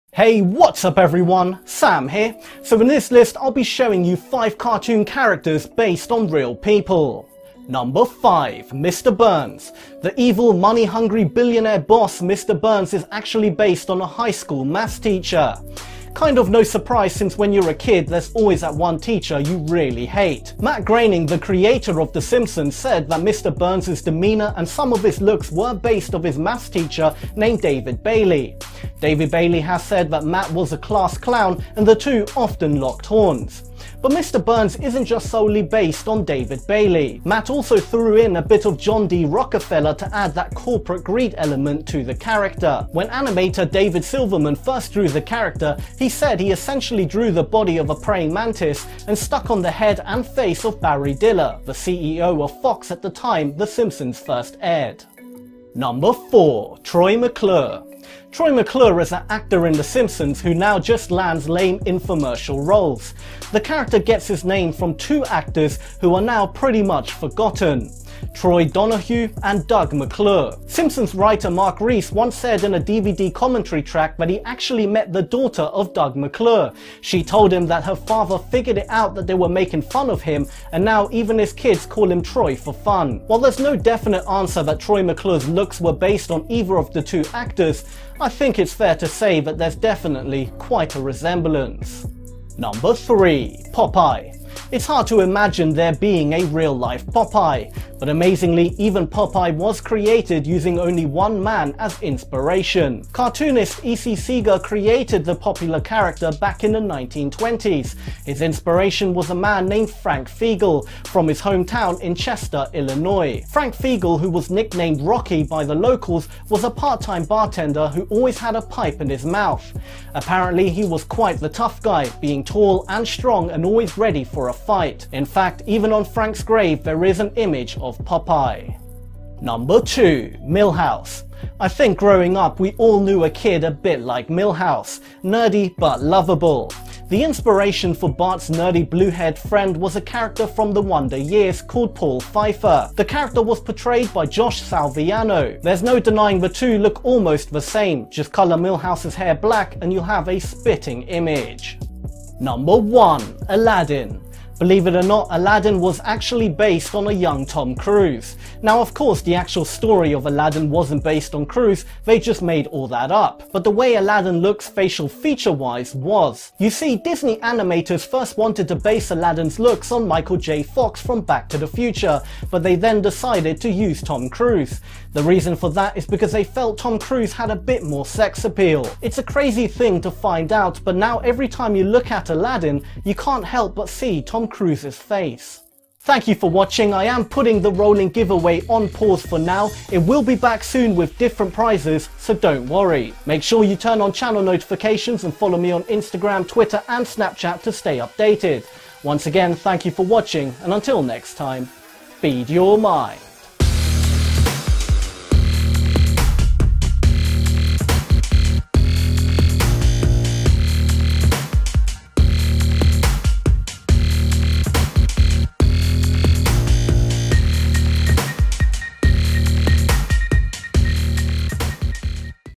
5 Cartoon Characters Based On Real People!
In this list I'll be showing you 5 cartoon characters based on real people.

5) Mr. Burns

The evil money hungry billionaire boss Mr.Burns is actually based on a high school maths teacher.
Kind of no surprise since when you're a kid there's alway that one teacher you hate.
Matt Groening the creator of The Simpsons said that Mr.Burns' demeanour and some of his looks were based on his math teacher named David Bailey.
David Bailey has said that Matt was a class clown and the two often locked horns.
But Mr.Burns isn't just solely based on David Bailey.
Matt also threw in a bit of John D. Rockefeller to add that corporate greed element to the character.
When animator David Silverman first drew the character, he said he essentially drew the body of a praying mantis and stuck on the head and face of Barry Diller, the CEO of Fox at the time the Simpsons first aired.

4) Troy McClure

Troy McClure is that actor in the Simpsons who now just lands lame infomercial roles.
The character gets his name from two actors who are now pretty much forgotten.
Troy Donahue & Doug McClure.
Simpsons writer Mike Reiss was said in a DVD commentary track that he actually met the daughter of Doug McClure.
She told him that her father figured it out that they were making fun of him and that now even his kids call him Troy for fun.
While there's no definite answer that Troy McClure's looks were based on either of these two actors, I think it's fair to say there's definitely quite a resemblance.

3) Popeye

It's hard to imagine there being a real life Popeye.
But amazingly even Popeye was created using only one man as inspiration.
Cartoonish E.C. Segar created the popular character back in the 1920's.
His inspiration was a man named Frank Fiegel from his home town in Chester, Illinois.
Frank Fiegel who was nicknamed Rocky by the locals, was a part time bartender who always had a pipe in his mouth.
Apparently, he was quite the tough guy, being tall & strong and always ready for a fight.
In fact, even on Frank's grave, there is an image of Popeye.

2) Milhouse Van Houten

I think growing up we all knew a kid a bit like Milhouse.
Nerdy but lovable.
The inspiration for Barts nerdy blue haired friend was a character from "The Wonder Years" called Paul Pfeiffer.
The character was portrayed by Josh Saviano.
There's no denying the two looks almost the same, just colour Milhouse's hair black and you'll have a spitting image.

1) Aladdin

Believe it or not, Aladdin was based on a young Tom Cruise.
Now, of course, the actual story of Aladdin wasn't based on Cruise, they just made that up,
But the way Aladdin looks facial feature wise was.
You see Disney animators first wanted to base Aladdins looks on Michael J. Fox from "Back to the future" but they then decided to use Tom Cruise.
The reason for that is because they felt Tom Cruise had a bit more sex appeal.
It's a crazy thing to find out but now every time you look at Aladdin you can't help but see Tom Cruise's face.